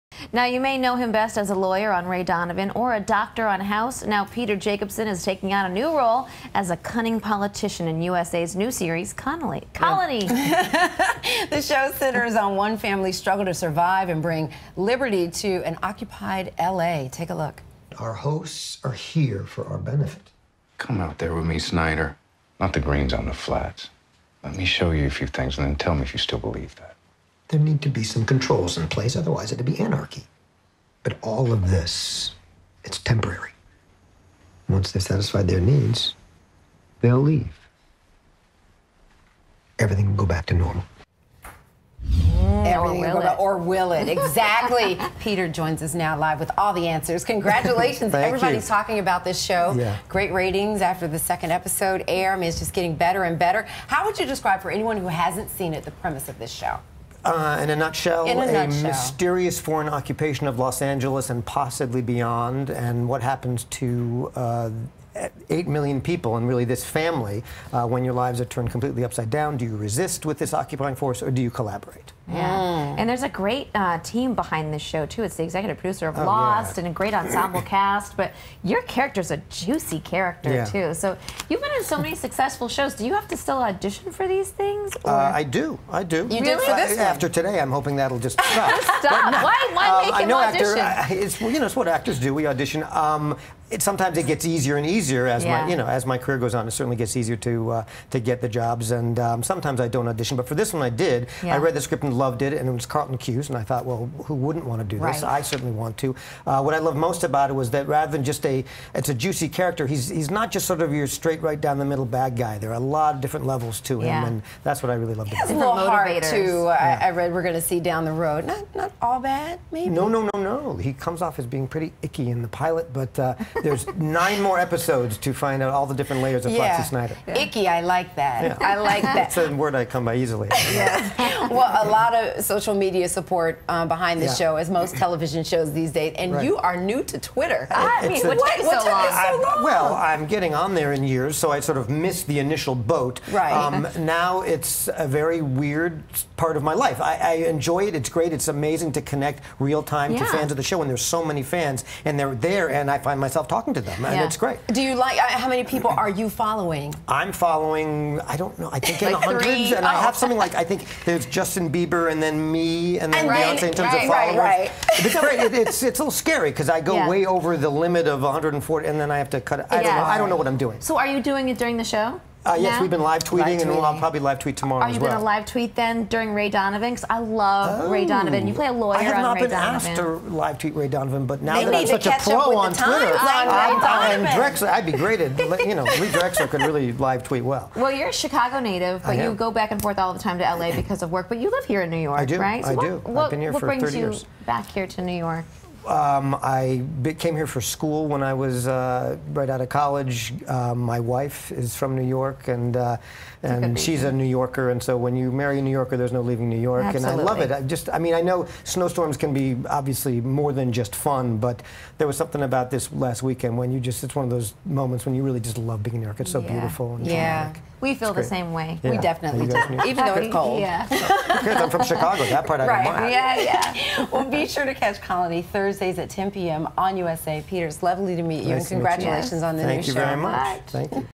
Peter Jacobson on "Colony"
Peter Jacobson stops by to chat about his buzzy new USA series "Colony."